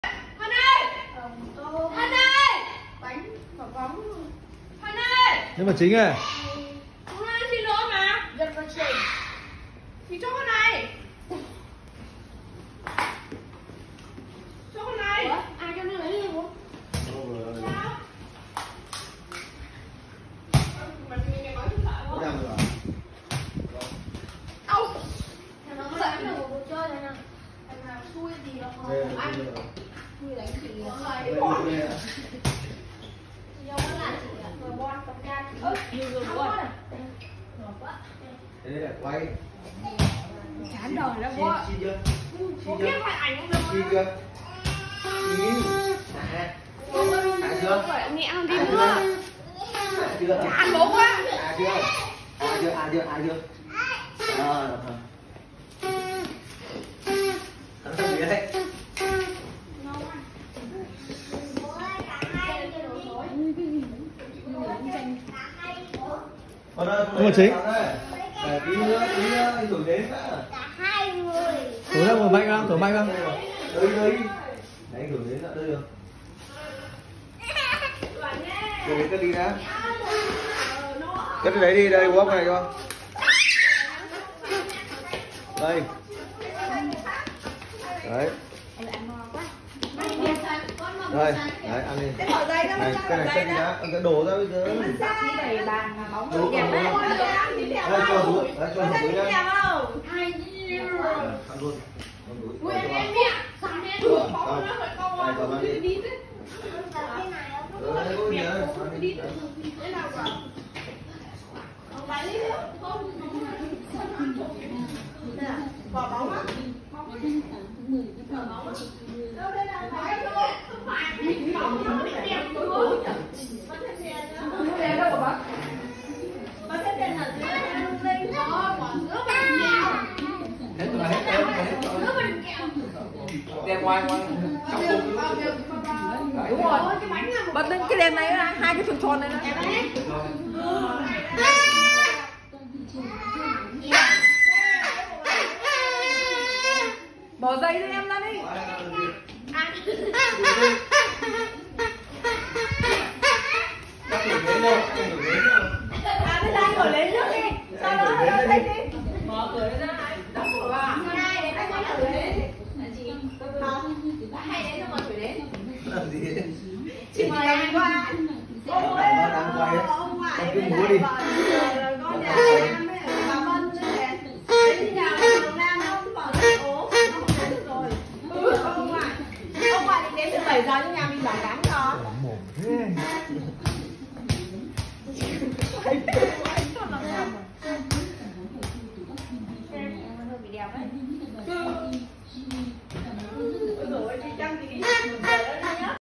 Sinh nhật Bonsai 2 tuổi
Ngày 29/10/2020 cả nhà đi ăn lẩu bò Úc rồi về nhà sinh nhật